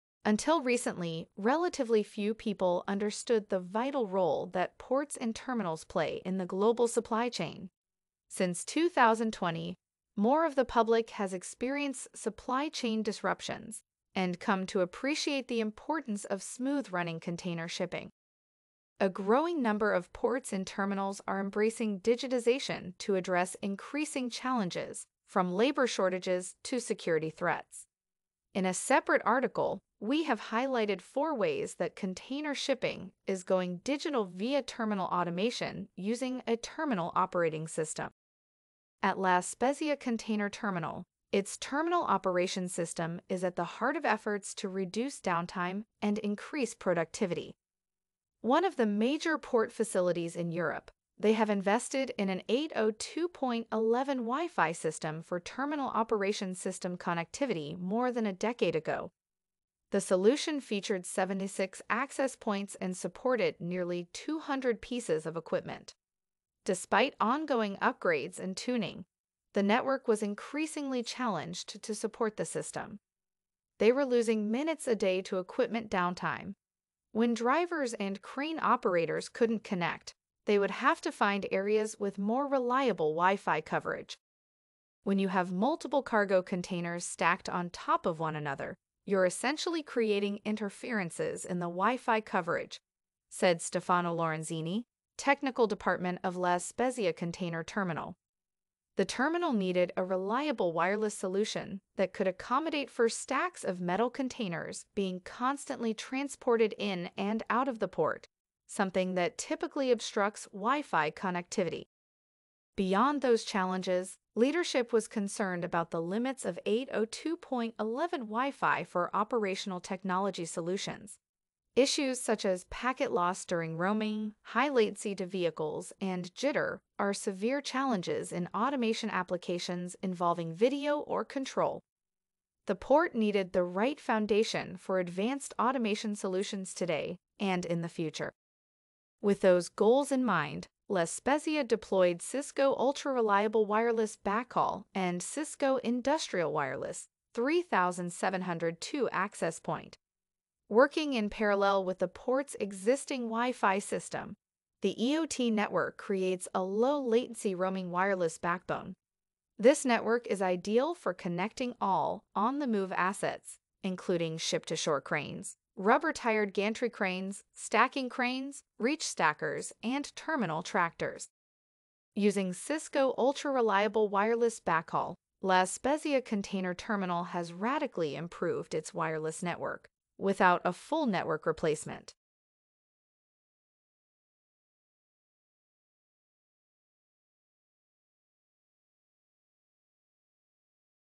IoT Network Delivers for Major European Port
Learn how Cisco IoT was able to assist Italy's La Spezia Container Terminal!

Visit now to read more about this!